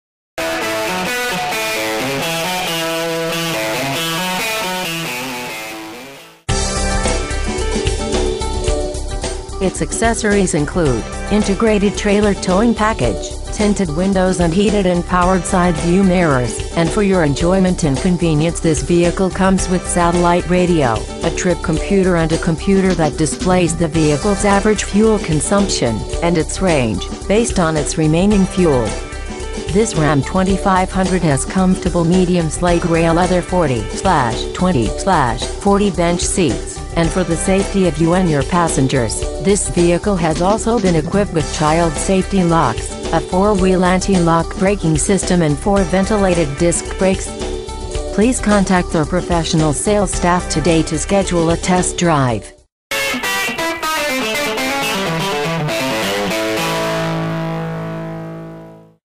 2009 Dodge Ram 2500 Truck Spring TX 77388
Year: 2009
 Make: Dodge
 Model: Ram 2500 Truck
 Engine: 6.7 liter inline 6 cylinder 12 valve

 Exterior: Patriot Blue